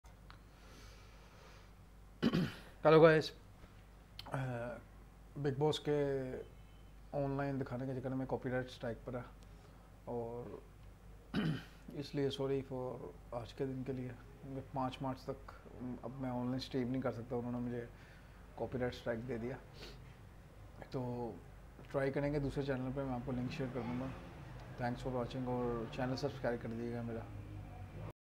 🔴LIVE : Bigg Boss COPY RIGHT STRIKE
beacuse of big boss today me get copy right strike sorry we cant do live today